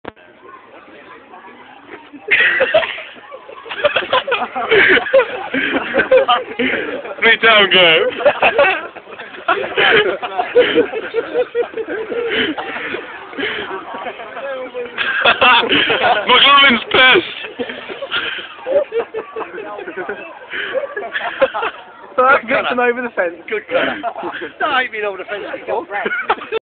Over He goes!
Matt T goin over the fleet town railings